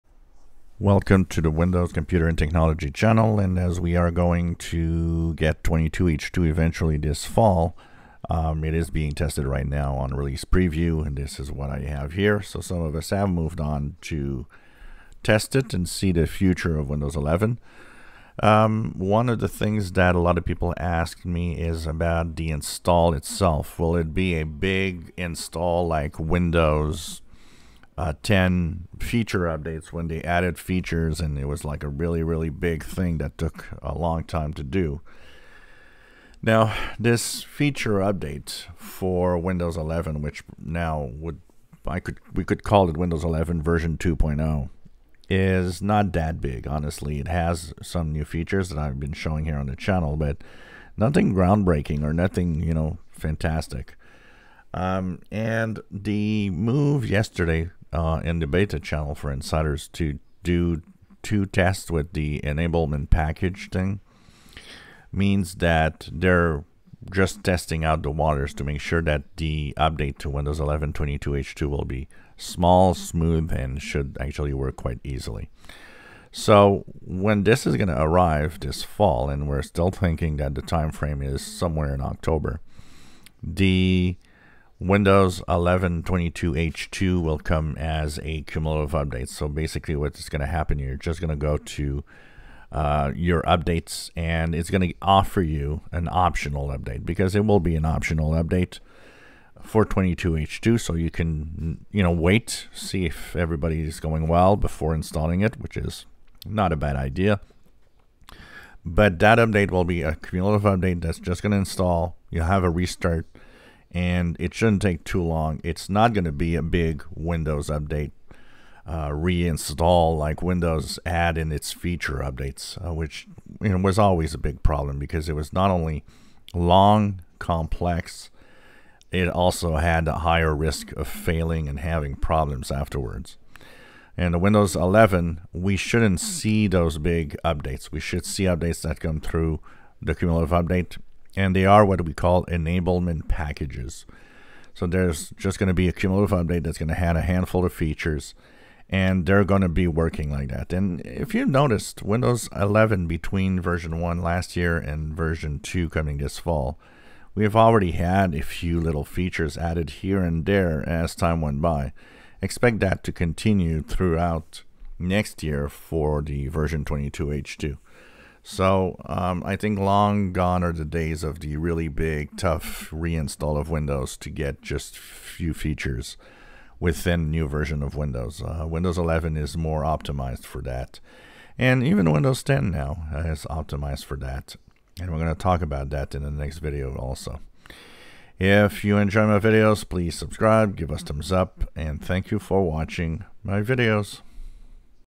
Windows 11 22H2 will be a small cumulative update rather than a big install
Over are the days of big long Windows re-installs due to a new feature update.  Welcome to the world of Enablement packages in Windows 11